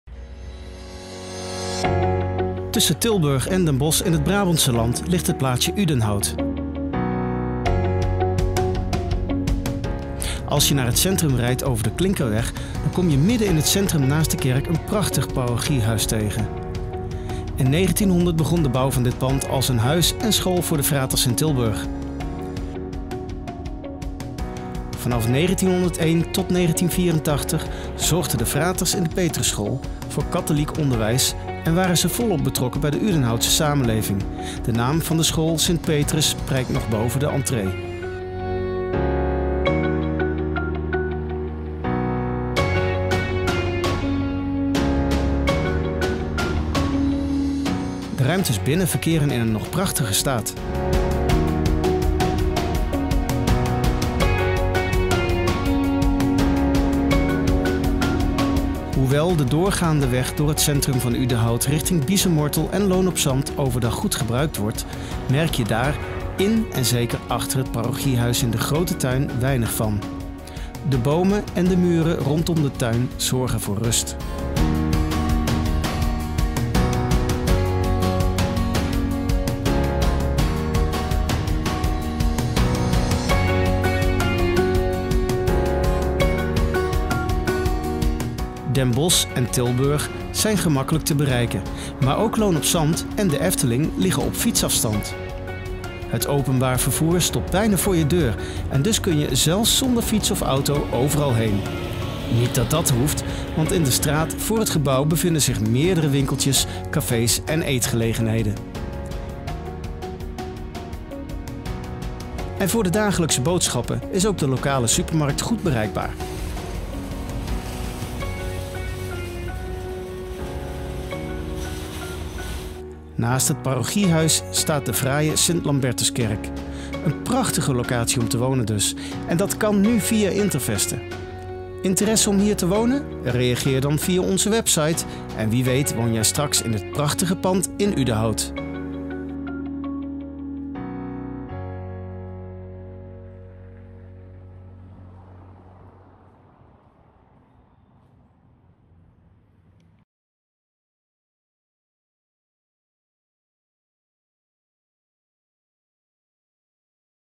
Woonruimte beschikbaar in Fraterhuis Udenhout
In het centrum van Udenhout staat dit prachtige pand uit het begin van de vorige eeuw. Het fraterhuis deed tot 1984 dienst als school voor vele mensen in en uit de omgeving. Na 1984 stond het bekend als opvanghuis voor uitgeprocedeerde asielzoekers en mensen die tijdelijk onderdak nodig hadden. Ook dat is nu gestopt. Om het pand niet leeg te laten staan komen er nu tijdelijke bewoners antikraak wonen. Het tijdelijke beheer wordt verzorgd door leegstandbeheerder Interveste. Heb jij interesse om in dit prachtige pand te wonen? Bekijk de woning dan snel op de website van Interveste om te zien of er nog ruimte beschikbaar is.